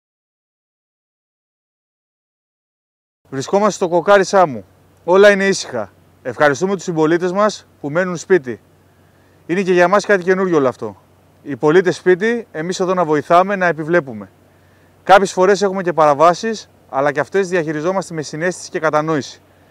ΚΟΙΝΩΝΙΚΟ ΜΗΝΥΜΑ ΑΣΤΥΝΟΜΙΚΗΣ ΔΙΕΥΘΥΝΣΗΣ ΣΑΜΟΥ ΒΡΑΔΥ ΚΟΚΚΑΡΙ
Κοινωνικό μήνυμα της Διεύθυνσης Αστυνομίας Σάμου για τα μέτρα αποφυγής άσκοπων μετακινήσεων για τον περιορισμό της διασποράς του κορονοϊού.